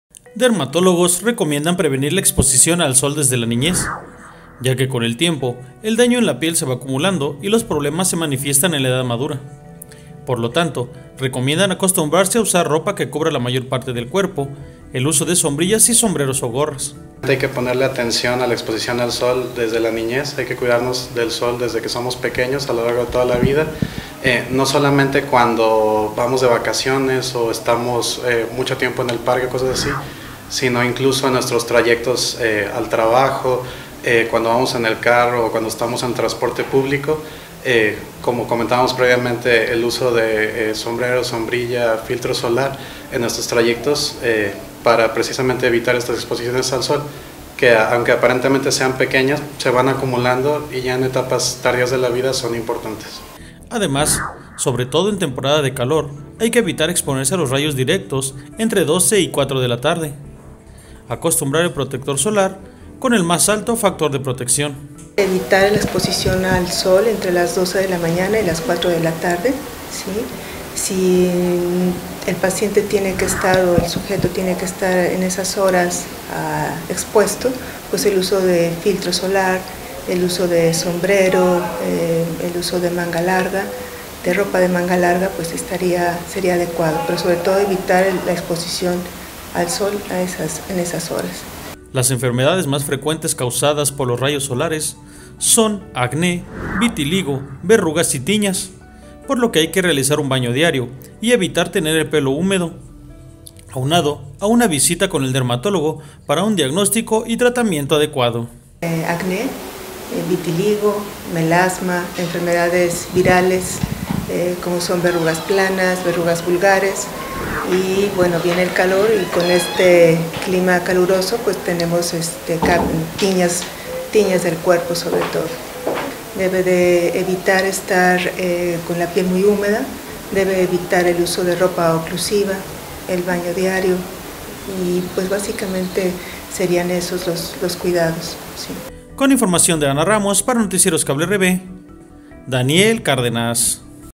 Recomiendan evitar exponerse al sol para evitar enfermedades en la piel
Dermatologos recomiendan evitar exponerse al sol por largos periodos para evitar enfermedades en la piel que a la larga podrian agravarse